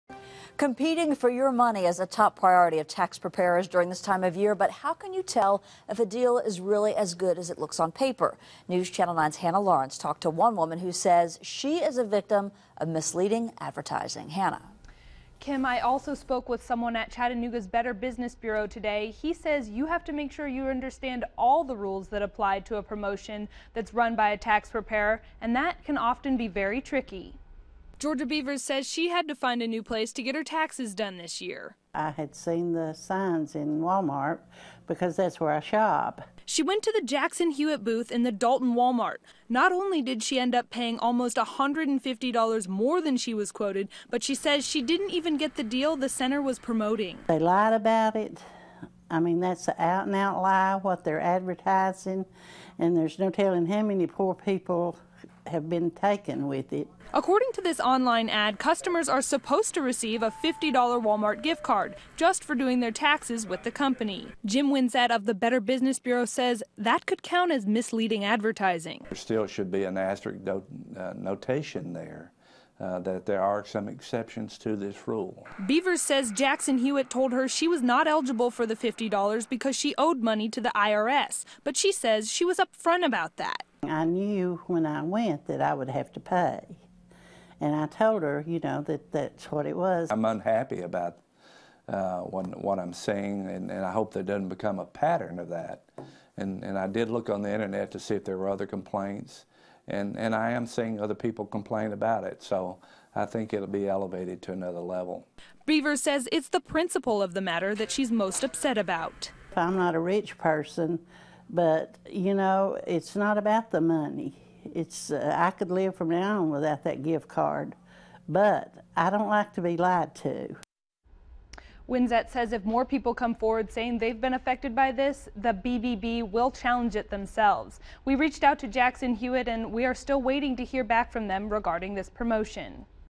Misleading Advertisements During Tax Return Season
Competing for your money is a top priority of tax preparers during this time of year- but some are saying it's tough to tell if a deal is really as good as it looks on paper.

Georgia Beavers says she had to find a new place to get her taxes done this year. She went to the Jackson-Hewitt booth in the Dalton Wal-Mart.

Not only did she end up paying almost $150 dollars more than she was quoted at the beginning of her session, but she says she also didn't get the deal the center is promoting.

"They lied about it. I mean that's an out and out lie what they are advertising and there's no telling how many people have been taken with it," Beavers said.

According to Jackson-Hewitt's online ad, customers are supposed to receive a $50 dollar Wal-Mart gift card just for doing their taxes with the company. Jim Winsett of the Better Business Bureau says this may be an instance of misleading advertising.

He says, "There should still be an asterisk notation there that there are some exceptions to this rule."

Beavers says after two weeks, Jackson-Hewitt told her she was not eligible for the $50 dollar gift card because she owed money to the IRS. But she says she was upfront about that, and it should not even matter to them.

"I says well what's that got to do with it? That's between me and the IRS," Beavers said.

Winsett says, "I'm unhappy about what i am seeing and i hope it's not a pattern. I did look online and I am seeing other people complain about it, so i think it will be elevated to another level."

Beavers says it's the principle of the matter that she's most upset about.

"I'm not a rich person but it's not about the money. I could live without the gift card, but I don't like being lied to."

Winsett says if more people come forward saying they are effected by this, the BBB will challenge it themselves. However, he says consumers should beware of buying into the ads before they read all the rules.

"Go into these things with eyes wide open as I like to say and understand the details and what you are agreeing to do," he says

NewsChannel 9 has reached out to Jackson-Hewitt and we are waiting to hear back from them regarding this promotion.